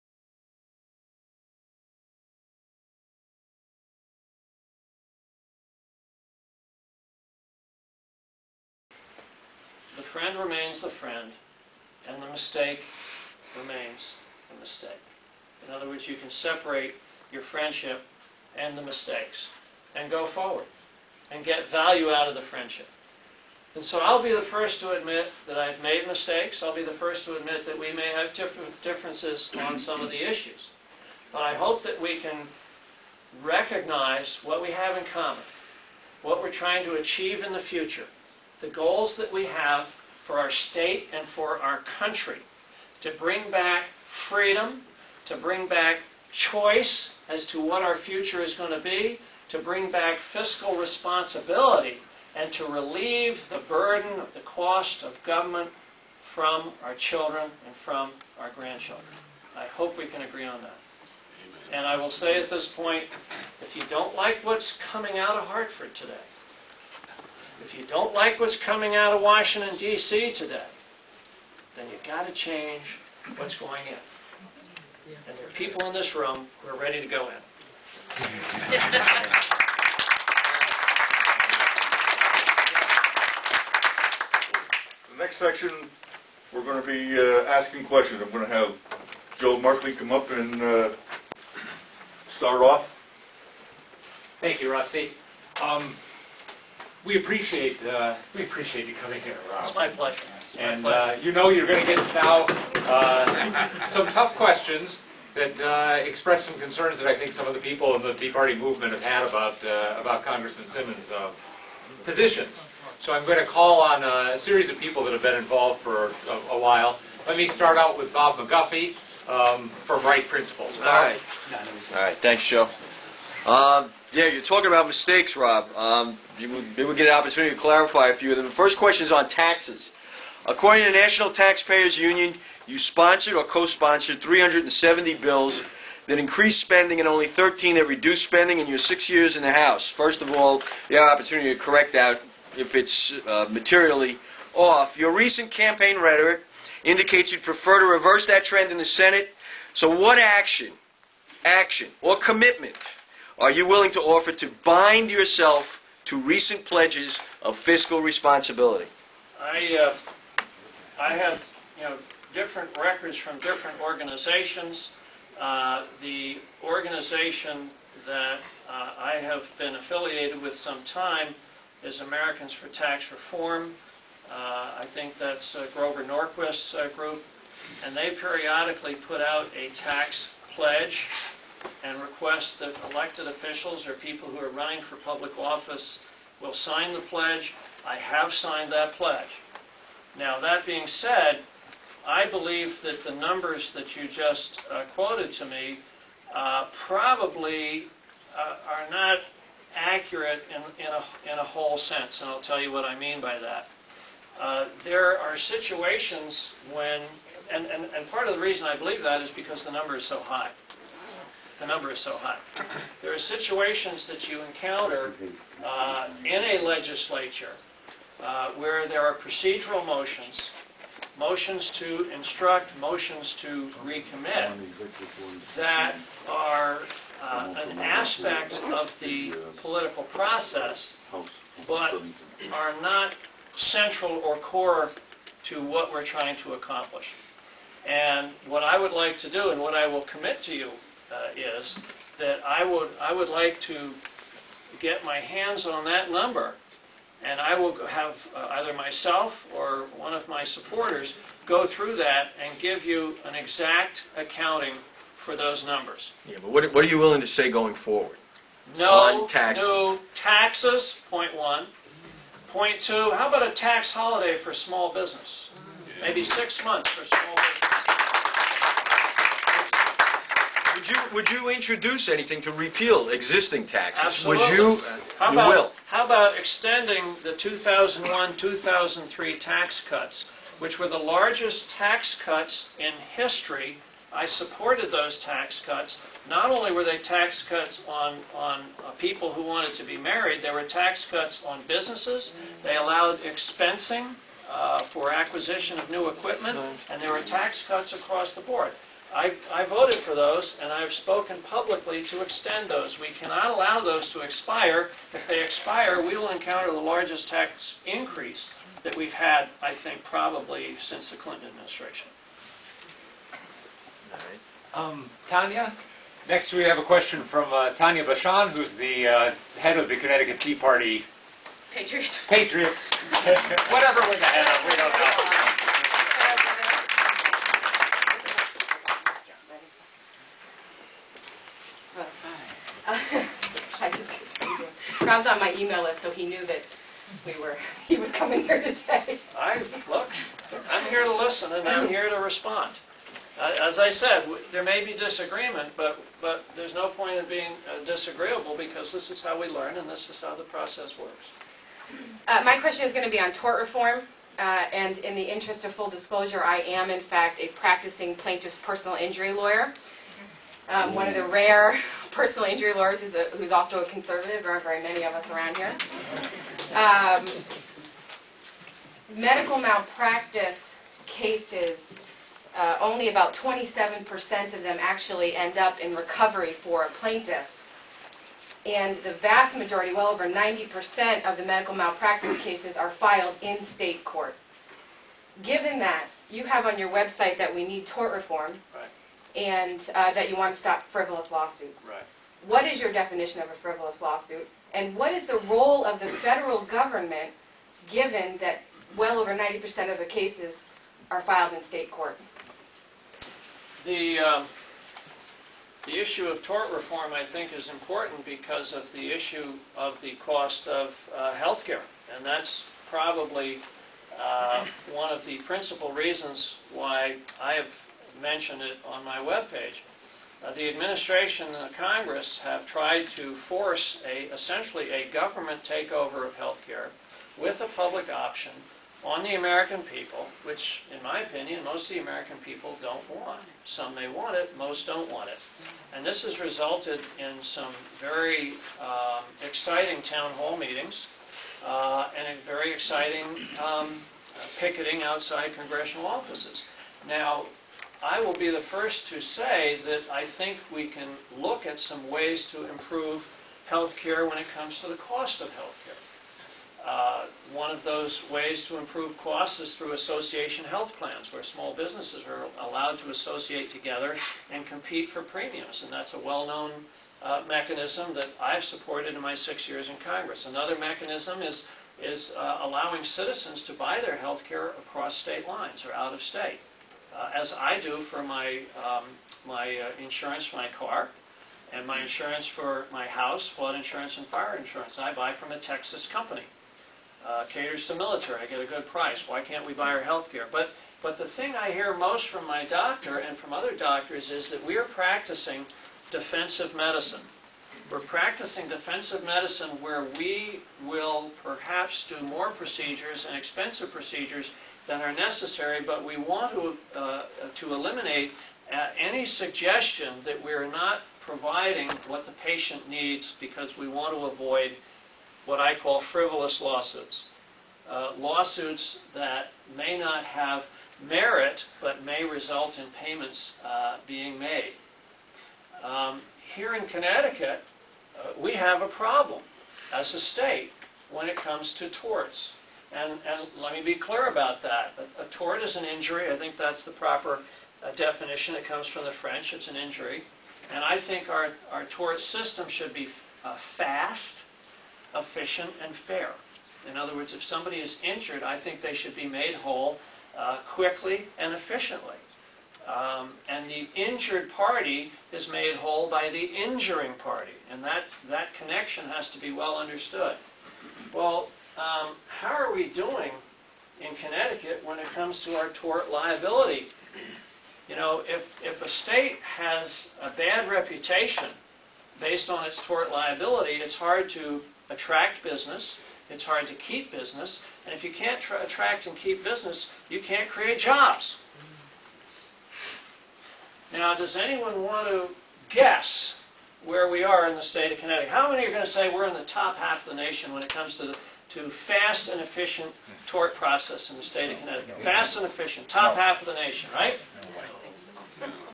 **Tea Party Patriots & Rob Simmons Part 3** Southington Feb 6, 2010.wmv
The Hartford Tea Party Patriots in conjunction with the alliance of Connecticut conservative grass roots groups, hosted the first in a series of candidate forums on Saturday, February 6, 2010. The meeting took place in a conference room at the Southington Holiday Inn Express. Former Congressman, Rob Simmons, is currently a candidate seeking the Republican nomination for Senate. He is one of four people hoping to oppose whom the Democratic Party picks to replace the retiring Chris Dodd. About 125 people attended the forum. After Simmon's introduced himself, they listened to a very informative question and answer session. The Tea Party Patriots try to educate, motivate and activate the public to better serve the state and nation. This forum was an education.